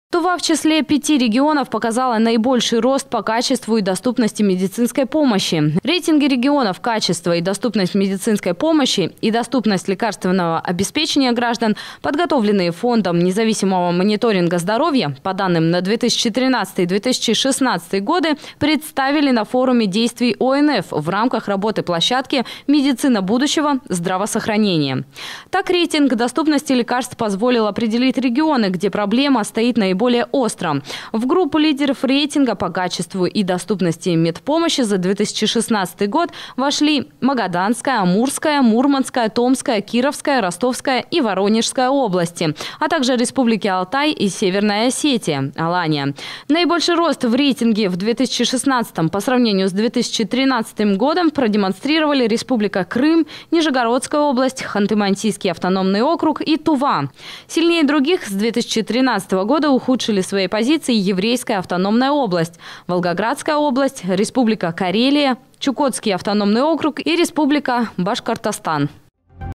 Качество и доступность медицинской помощи в Туве
Тува в числе пяти регионов показала наибольший рост по качеству и доступности медицинской помощи. Рейтинги регионов «Качество и доступность медицинской помощи» и «Доступность лекарственного обеспечения граждан», подготовленные Фондом независимого мониторинга «Здоровье» по данным за 2013–2016 гг., представили на «Форуме Действий ОНФ» в рамках работы площадки «Медицина будущего: здраво Сохранение».